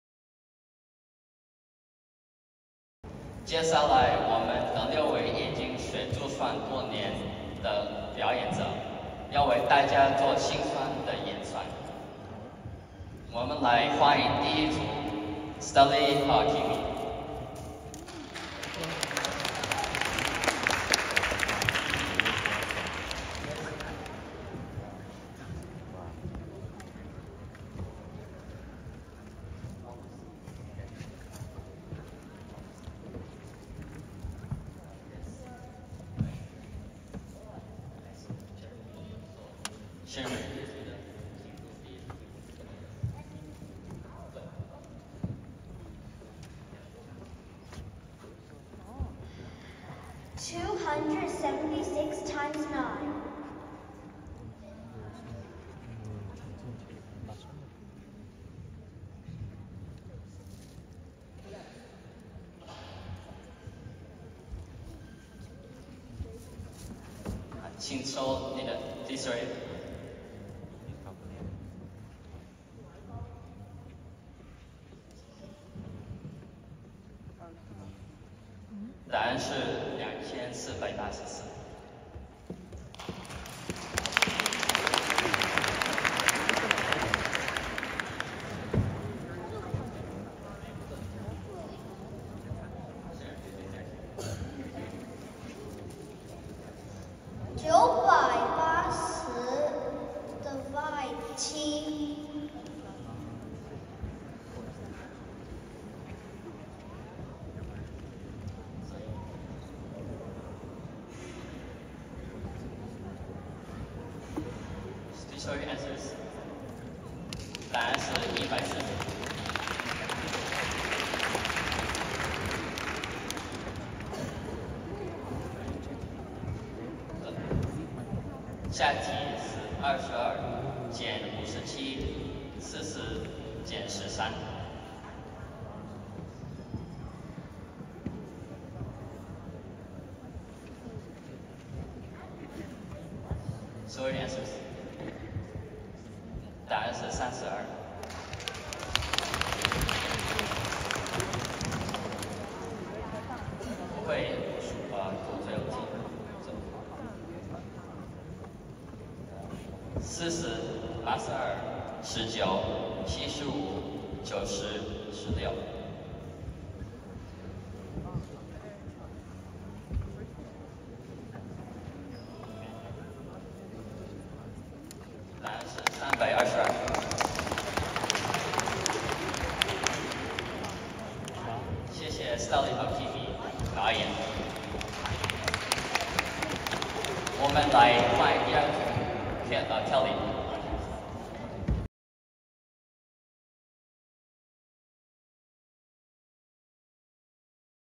Stelly Fong Vlog : Mental Math (Abacus Culture) Skill Demo
Back in early February, I was invited to attend an event celebrating 2023 Lunar New Year by the Longriver Chinese School @ Fremont, CA.

It was certainly exciting, I get to practice and gaining experience to perform mental math (abacus culture) on the stage. It was a great opportunity to express my math skill to inspire other kids and to motivate them to learn abacus.

1:13 276 x 9 = 2484
1:43 980 / 7 = 140
2:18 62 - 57 + 40 - 13 = 32
2:51 40 + 82 + 19 + 75 + 90 + 16 = 322

Special thanks dedicated to Chinese American Abacus Association (CAAA) for the opportunity.